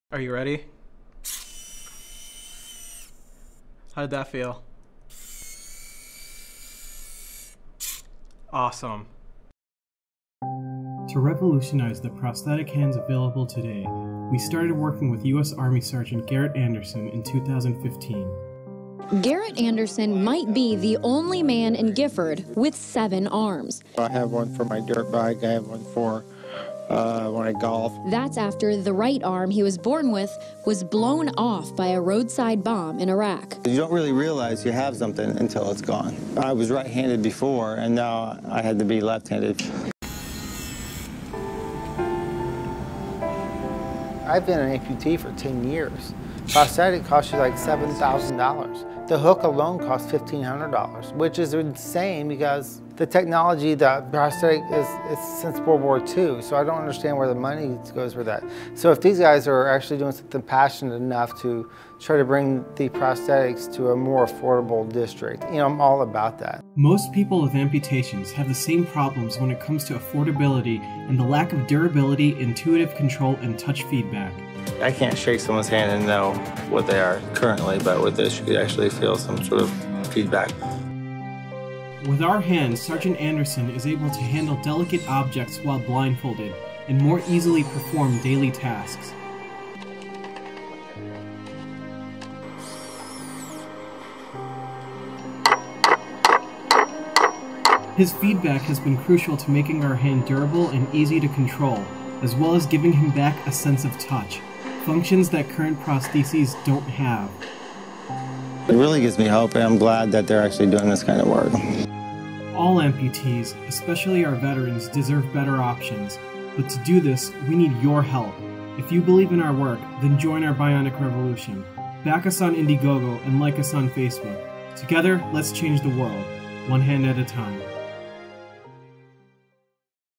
Sgt. Garrett Anderson's Story - PSYONIC
Help 11.4 million people with amputations just like Sgt. Anderson.

Ret. US Army Sgt. Garrett Anderson lost his right arm due to a roadside bomb in Iraq. PSYONIC is working with him to build an affordable, advanced bionic arm with sensory feedback.